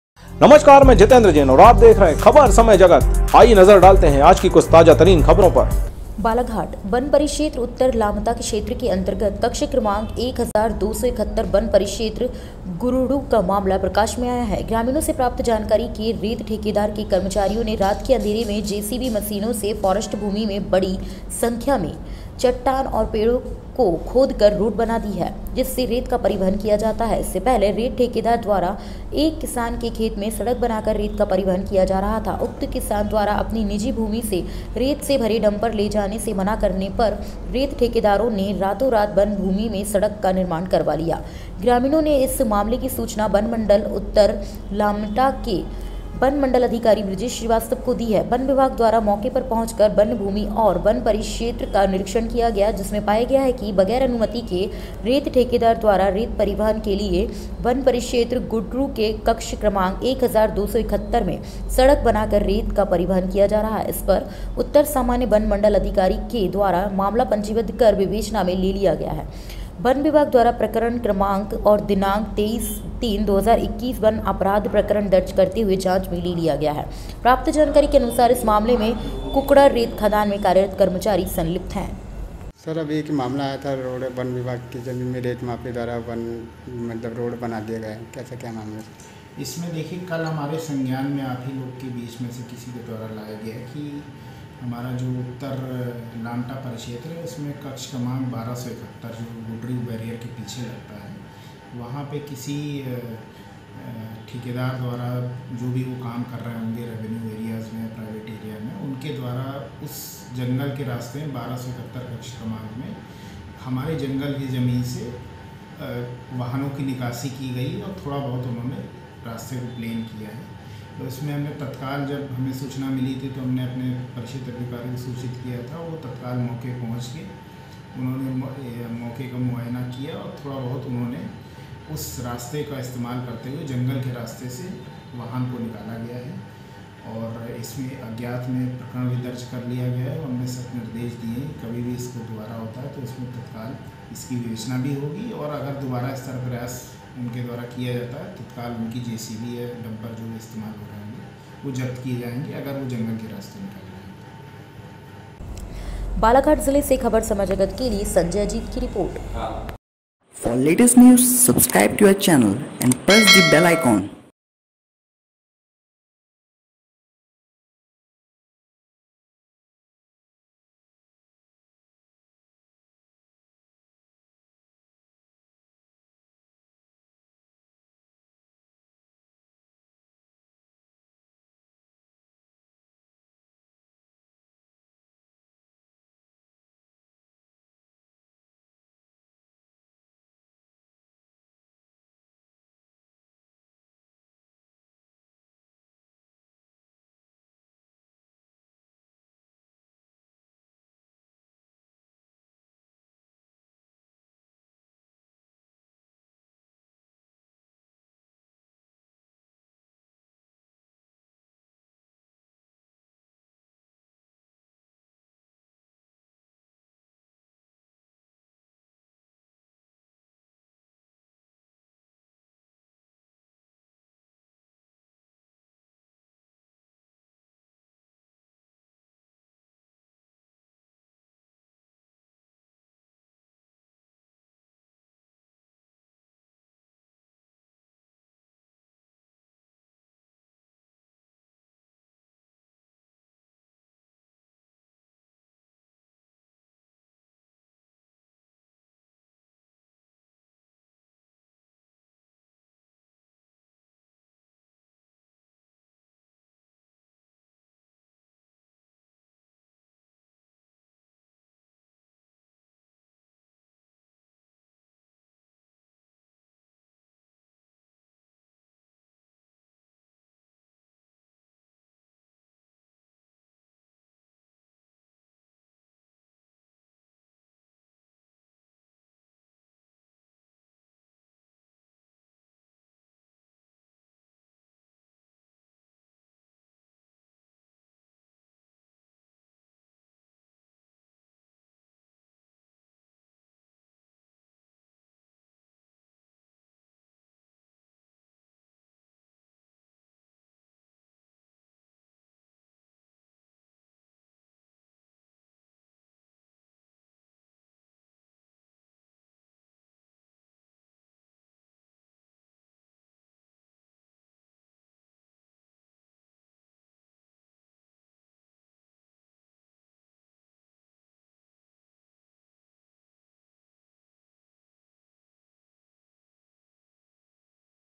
बालाघाट - रेत माफिया ने फॉरेस्ट की भूमि में रातो रात बना डाली सड़क
बालाघाट जिले से संजय अजित की रिपोर्ट